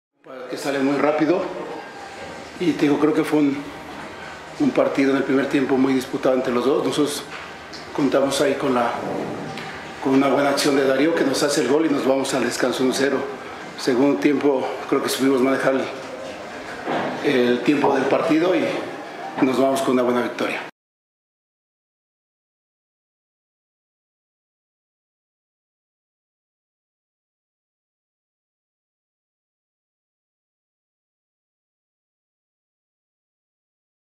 Conferencia de prensa Ignacio Ambriz Pachuca 0-3 América
Declaraciones de nuestro entrenador después de la victoria en el Estadio Hidalgo